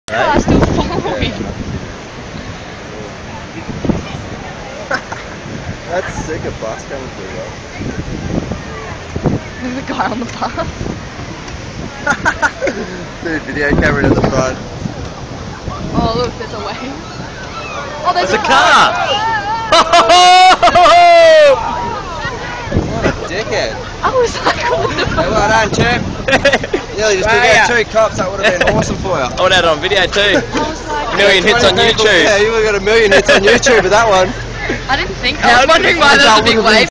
Alex Heads Foam Car Surprise!
Car unexpectantly appears out of the foam created by the ocean sea water & ALMOST cleaned up the Cops waiting right up front!!

This was at Alexandra Headlands in Queensland 27/01/2013